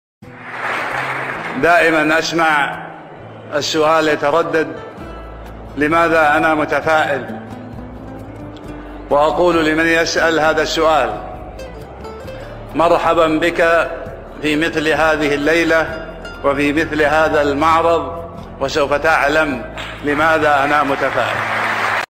صاحب السمو الأمير خالد الفيصل | لماذا انا متفائل
صاحب السمو الأمير خالد الفيصل فى معرض شباب الأعمال - جدة، المملكة العربية السعودية